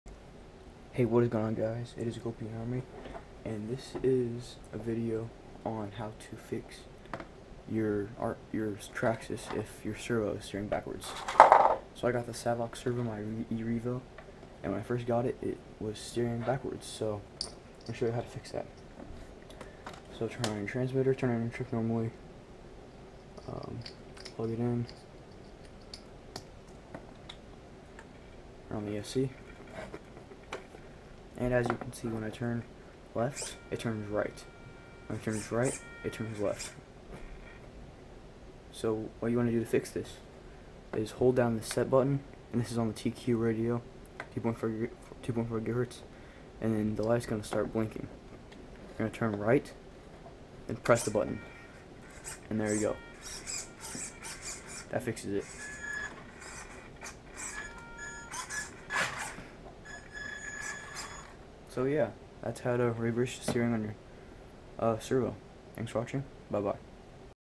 How to reverse a servo with the Traxxas TQ 2.4GHz radio!
step 1- connect radio to your traxxas
step 2- press and hold down the set button till it blinks
step 3- turn your wheels right
step 4- as you are turning the wheels press the set button.